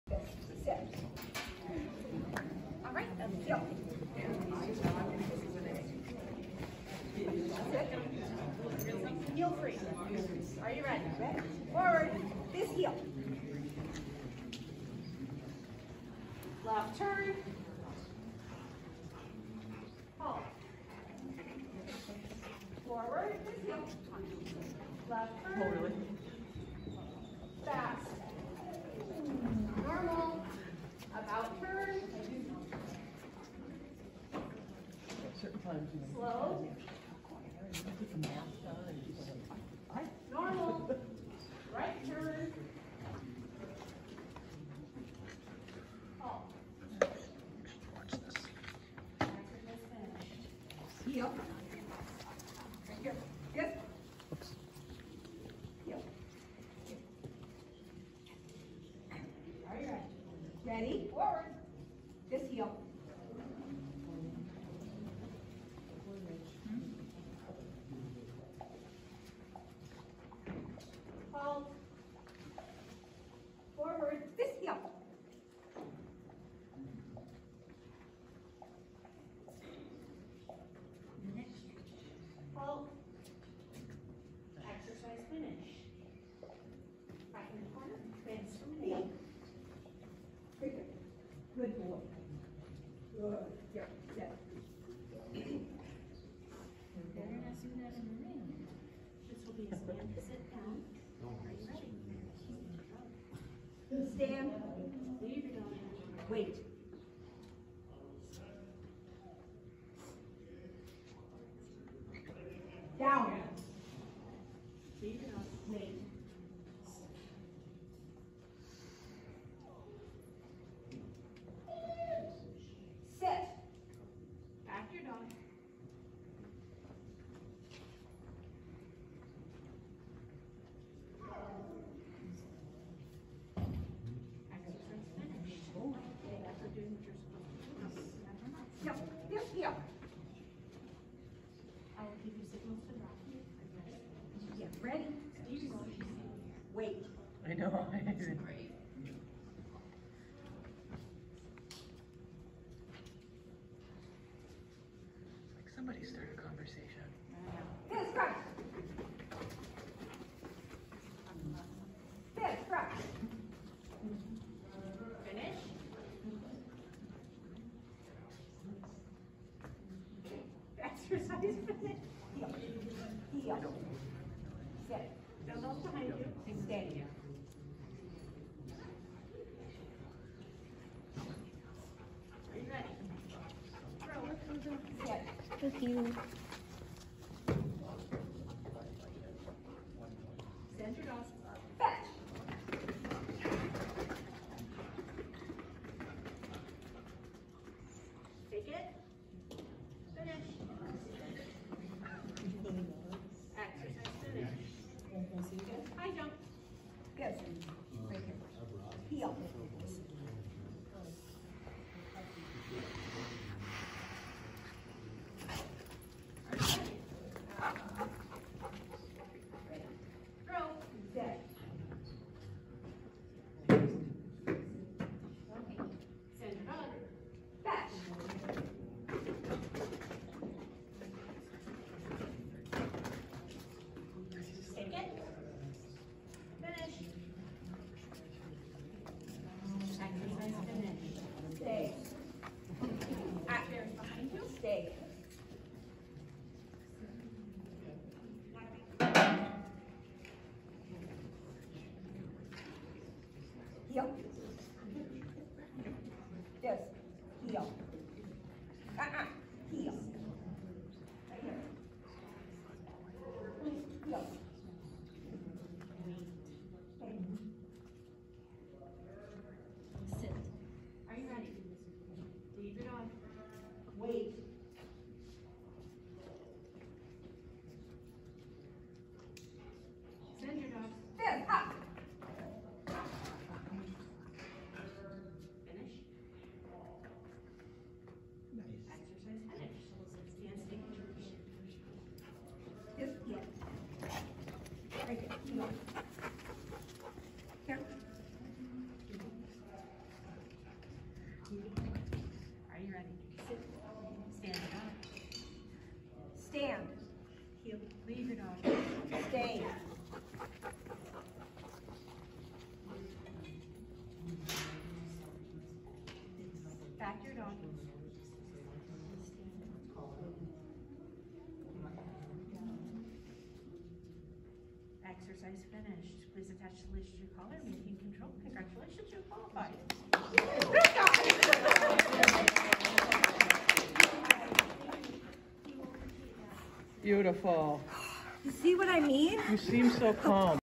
AKC Obedience Trial PHz Open B 2/6/22
1st time in Open B at Cambridge Kennel Club at Total Recall